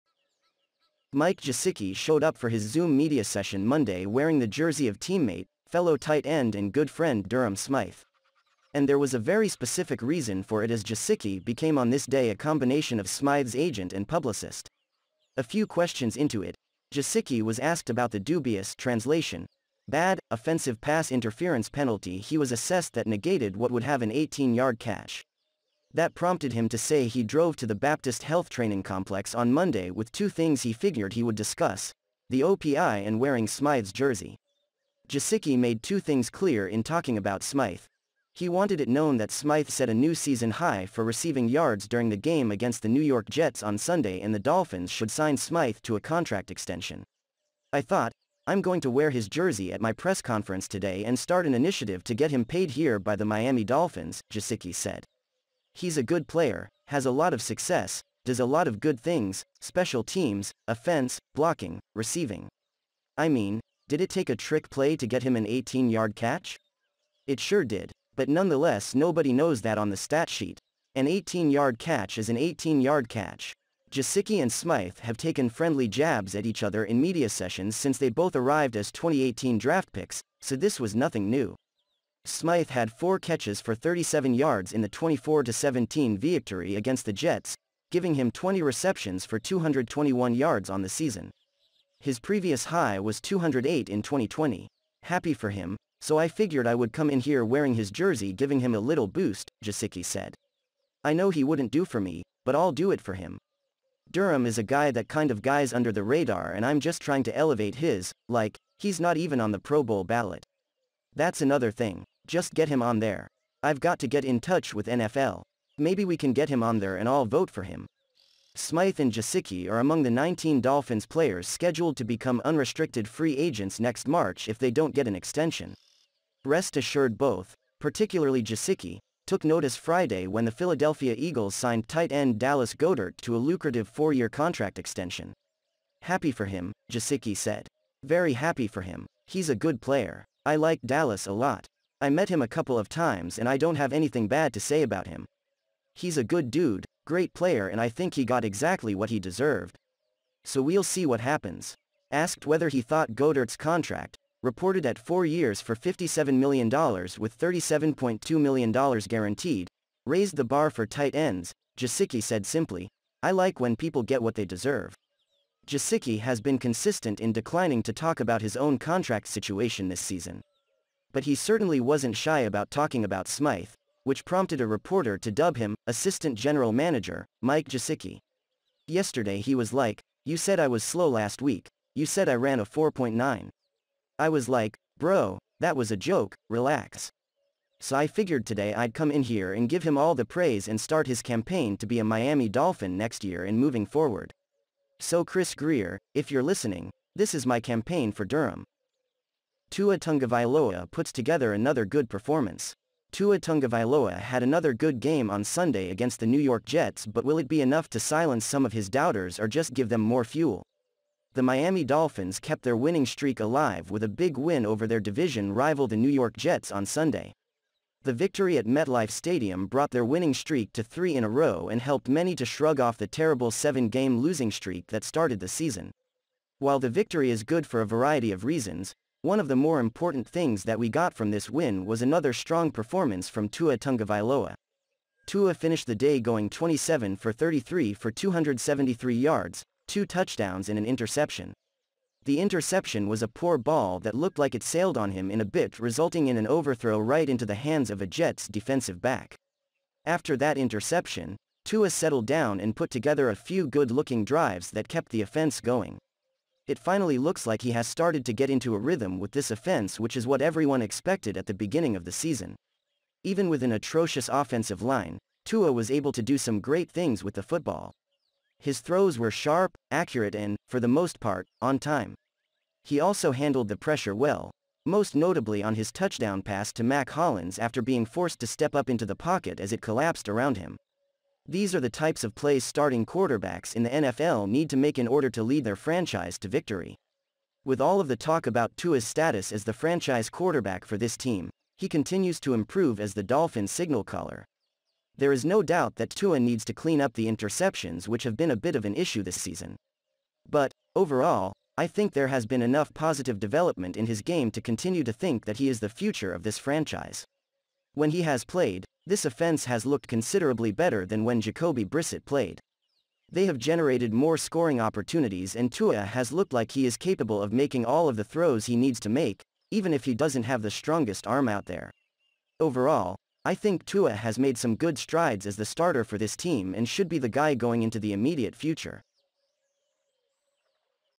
Miami Dolphins News| Gesicki Campaigns for Smythe. Tua puts together another good performance
00:00 Gesicki Campaigns for Smythe.
3:19 QB Review: Tua Tagovailoa puts together another good performance.